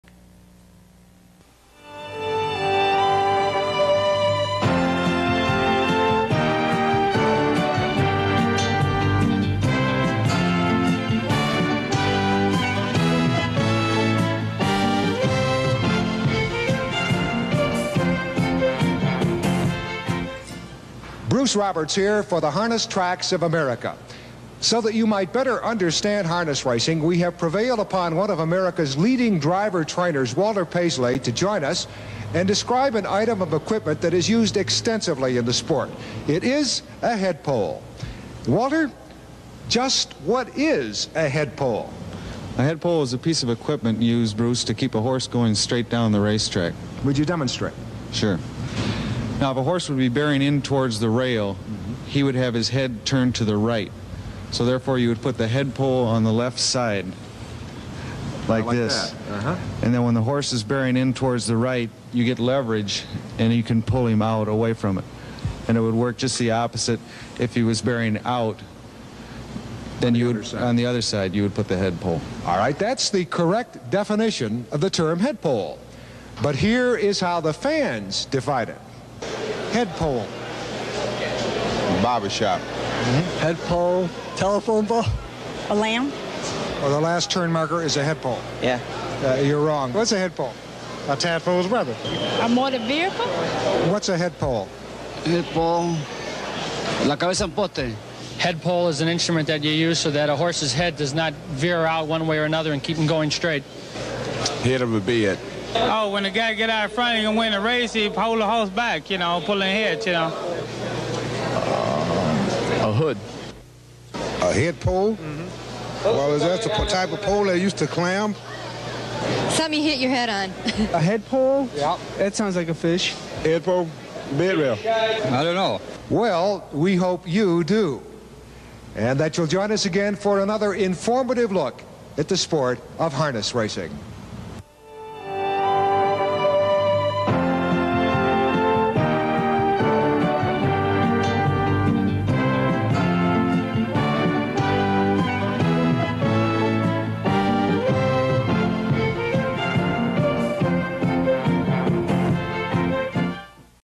Monticello Raceway Presents Walter Paisley in Track Topics "What is a Headpole ?"
Harness Driver Walter Paisley explains what a headpole is and then fans give their answers.